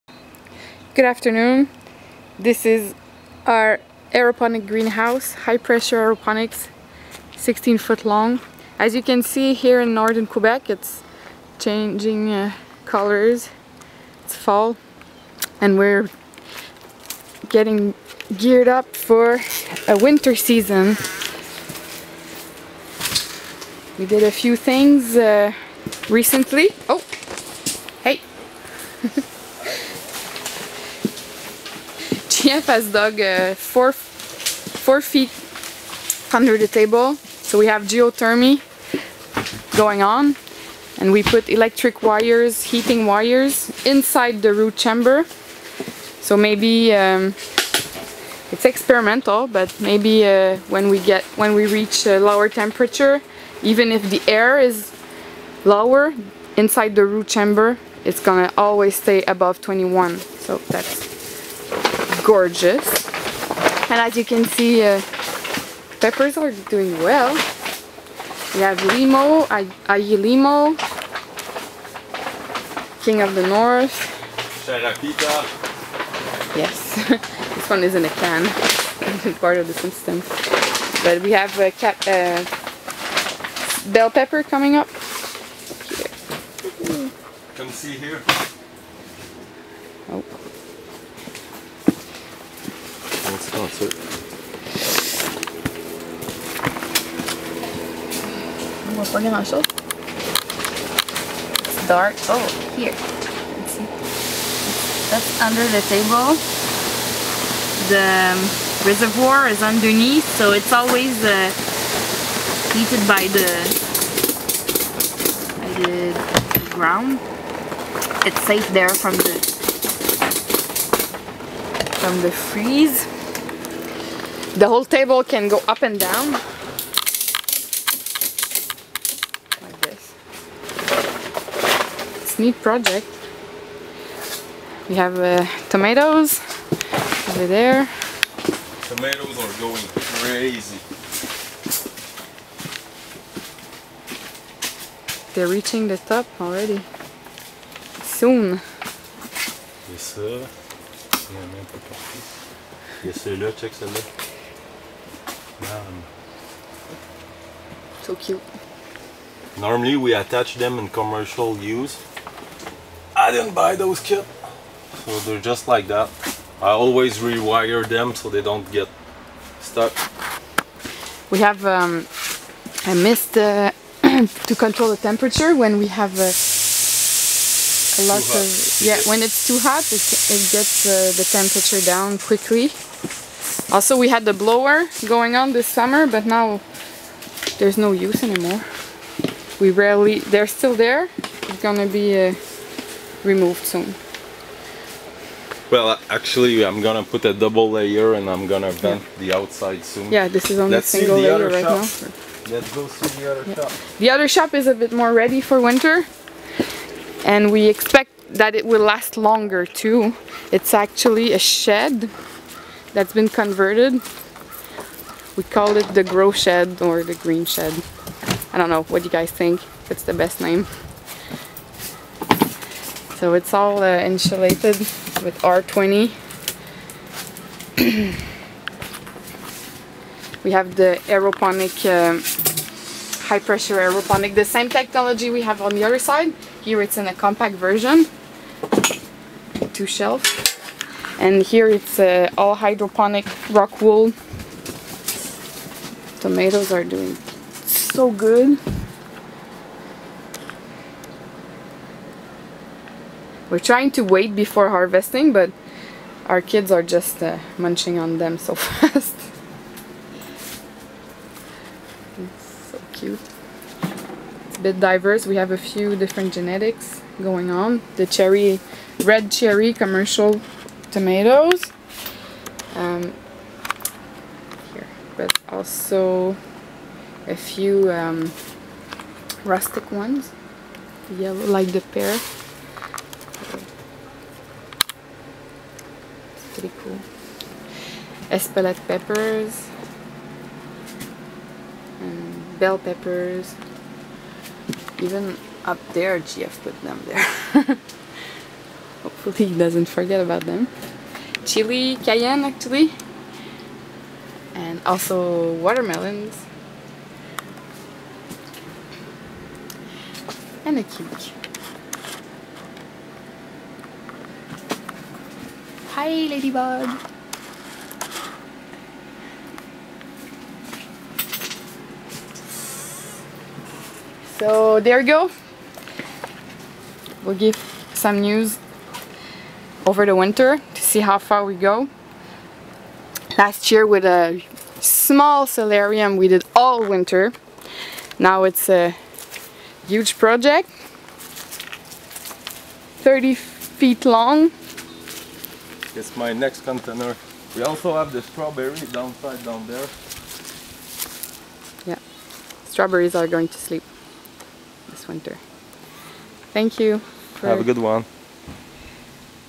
High Pressure Aeroponic Greenhouse full of peppers
Our largest High Pressure Aeroponic table so far 16 feet long in a geothermal wooden Greenhouse.  Also featuring the Growshed or Greenshed (please leave your favorite term in the comment) ready for a Canadian winter.